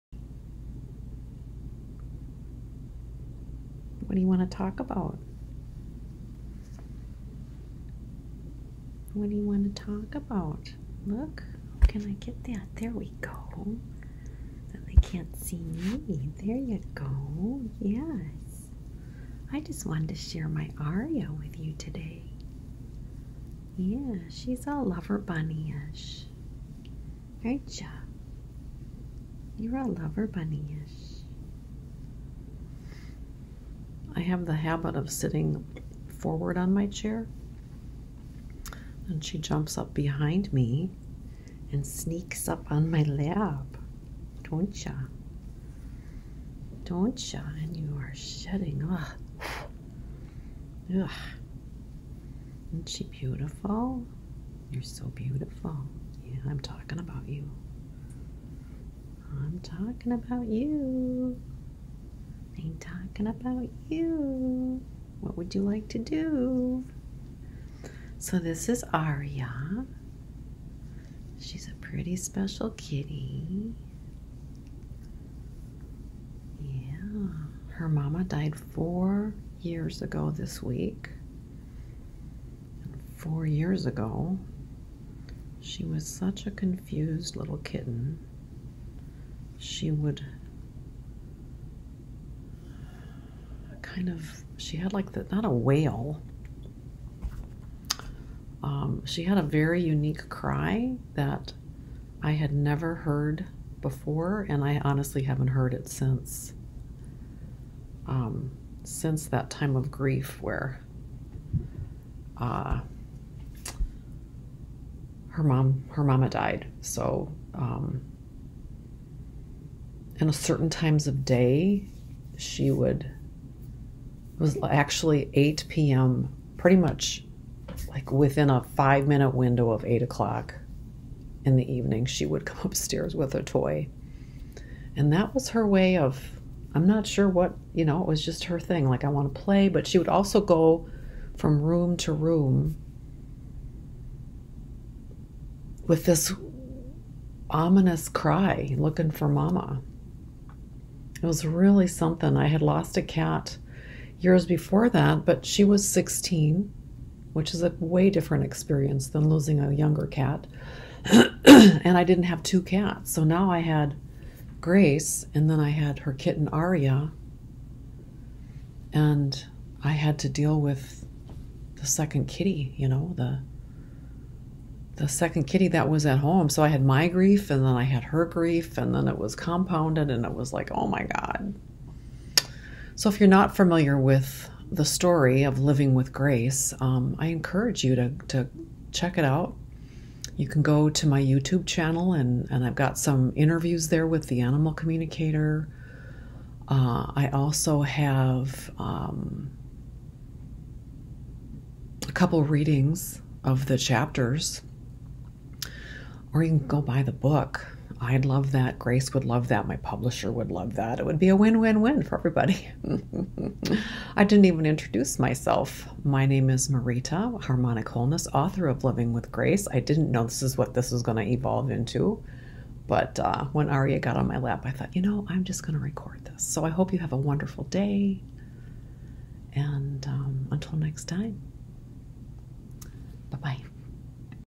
4 Years Later - Living Without Grace
Four years after her mama Grace's death, I had a little conversation with Aria and you! She's a full grown cat now. She has a few scars from seeing her mama suffer. Come see how beautiful she is!